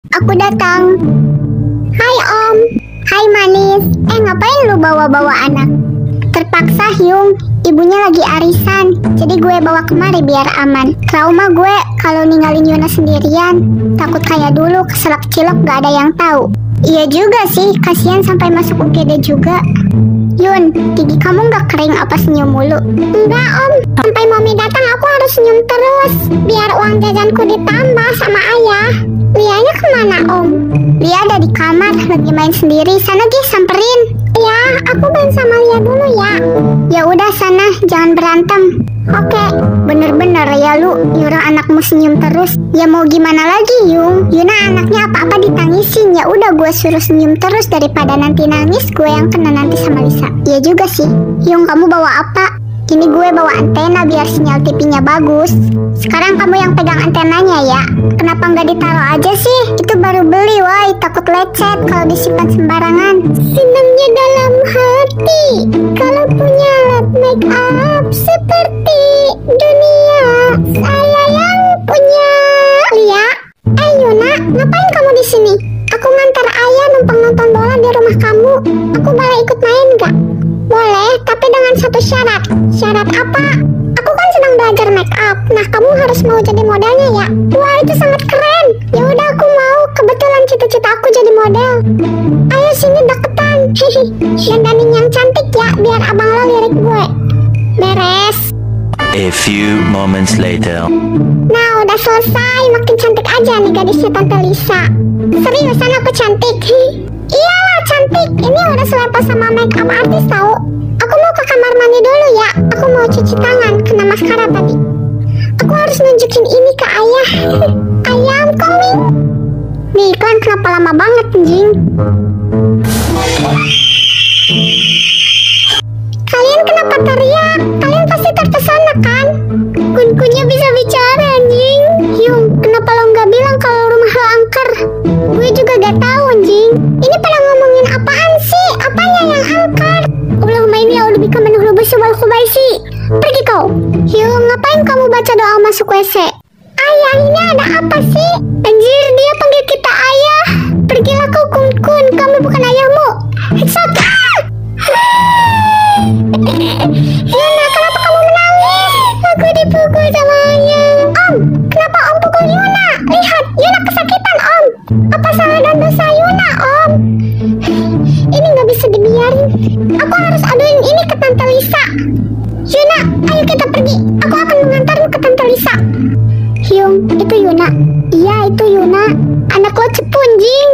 Gara-gara Make Up (Part 1) | Riasan yang membuat semua orang kaget
Just for fun!!